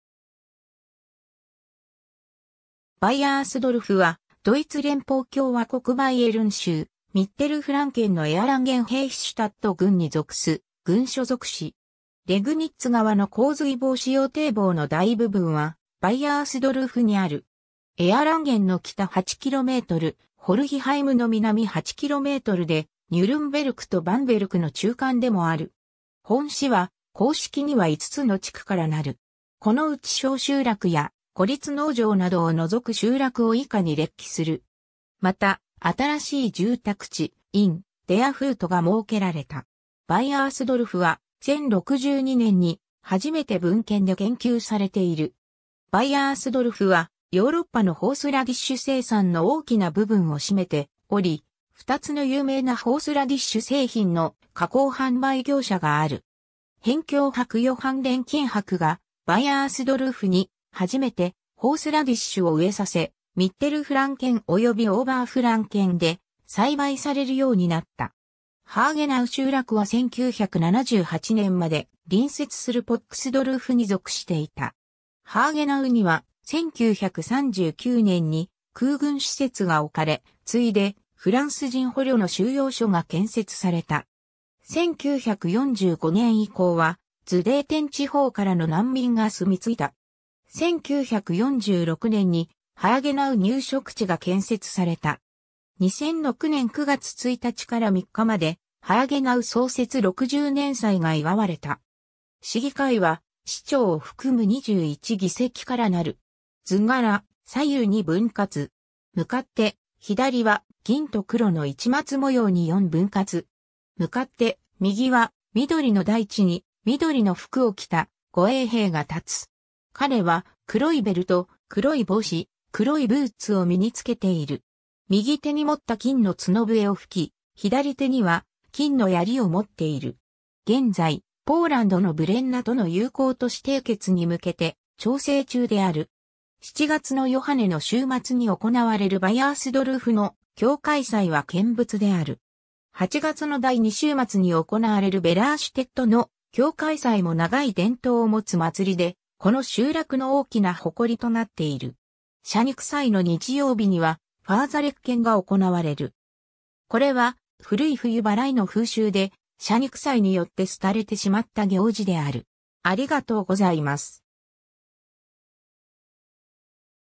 バイアースドルフ
エアランゲン＝ヘーヒシュタット郡
バイアースドルフ (ドイツ語: Baiersdorf, ドイツ語発音: [ˈba‿jərsdɔrf]) は、ドイツ連邦共和国バイエルン州ミッテルフランケンのエアランゲン＝ヘーヒシュタット郡に属す郡所属市。
 レグニッツ川の洪水防止用堤防（500m）の大部分はバイアースドルフにある。
エアランゲンの北8km、フォルヒハイムの南8kmで、ニュルンベルクとバンベルクの中間でもある。
 本市は、公式には5つの地区 (Ort) からなる。
 また、新しい住宅地「イン・デア・フート」が設けられた。
 バイアースドルフは、1062年に初めて文献で言及されている。
 バイアースドルフは、ヨーロッパのホースラディッシュ生産の大きな部分を占めており、2つの有名なホースラディッシュ製品の加工販売業者がある。
辺境伯ヨハン錬金伯 (1401年–1464年)が、バイアースドルフに初めてホースラディッシュを植えさせ、ミッテルフランケンおよびオーバーフランケンで栽培されるようになった。
 ハーゲナウ集落は1978年まで、隣接するポックスドルフに属していた。
ハーゲナウには、1939年に、空軍施設がおかれ、次いでフランス人捕虜の収容所が建設された。
1945年以降はズデーテン地方からの難民が住み着いた。
1946年にハーゲナウ入植地が建設された。
2006年9月1日から3日までハーゲナウ創設60年祭が祝われた。
 市議会は、市長を含む21議席からなる。
 図柄: 左右二分割。
向かって左は、銀と黒の市松模様に四分割。
向かって右は、緑の大地に緑の服を着た護衛兵が立つ。
彼は、黒いベルト、黒い帽子、黒いブーツを身につけている。
右手に持った金の角笛を吹き、左手には金の槍を持っている。
 現在、ポーランドのブレンナとの友好都市締結に向けて調整中である。
 7月のヨハネの週末に行われるバイアースドルフの教会祭は見物である。
8月の第2週末に行われるヴェラーシュテットの教会祭も長い伝統を持つ祭りで、この集落の大きな誇りとなっている。
謝肉祭の日曜日には、ファーザレッケン (Fasalecken) が行われる。
これは、古い冬払いの風習で、謝肉祭によって廃れてしまった行事である。